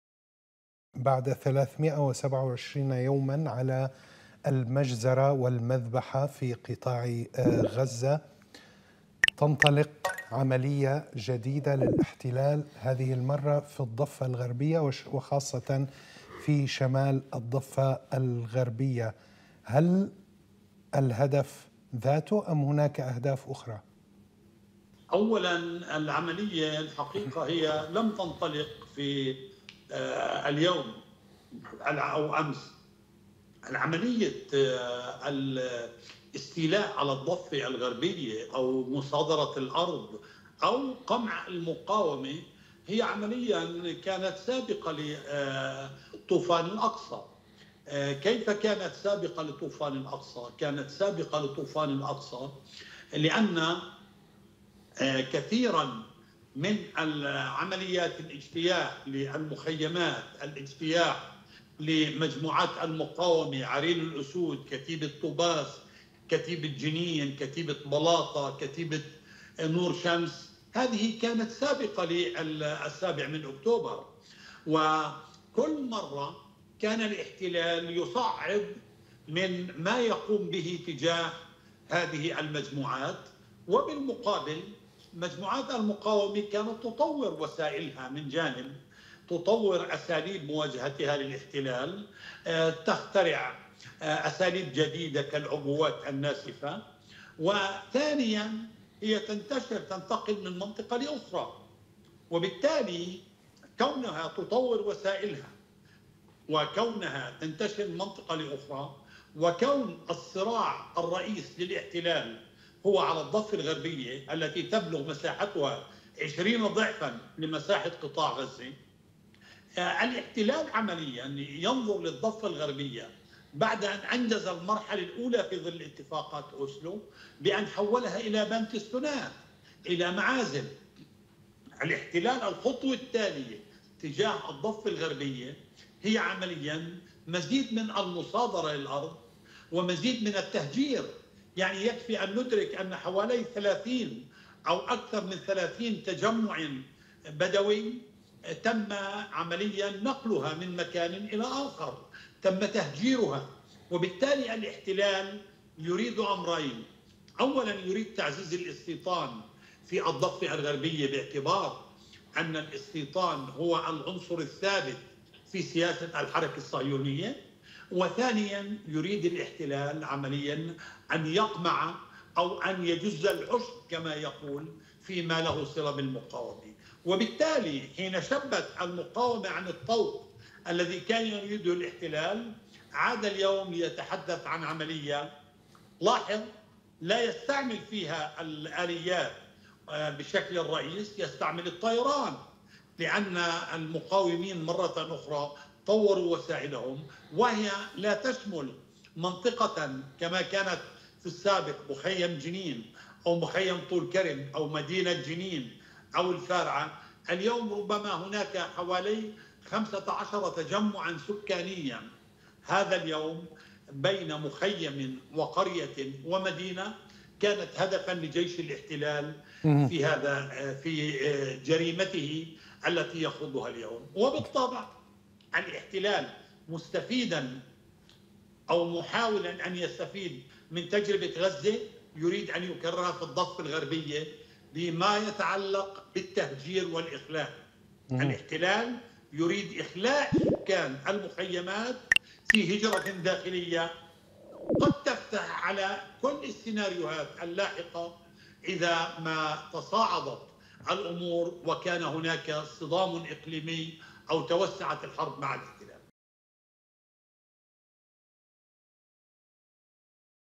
الباحث السياسي عمر عساف: نية الاحتلال الاستيلاء على الأرض في الضفة سابقة للطوفان
هل العملية العسكرية الإسرائيلية في شمال الضفة الغربية محدودة أم مقدمة لعملية واسعة وطويلة؟
كيف يتواجه الفلسطينيون في الضفة الغربية الاحتلال الإسرائيلي والاقتحامات اليومية؟
لماذا لم تتوقف إسرائيل عن اقتحام الضفة الغربية بشكل يومي؟

تغطية خاصة | 28 أغسطس 2024 | زياد السيد يستضيف الكاتب والمحلل السياسي، عمر عساف.